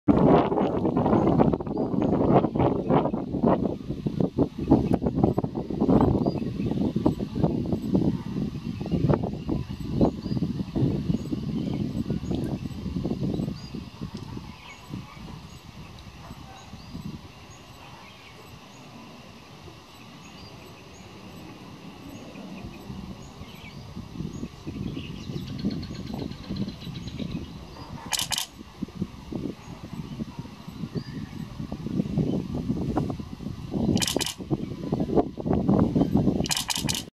Takeaway Meal
Mangrove Kingfisher catching a crab at low tide at Boondall Wetlands QLD 280815.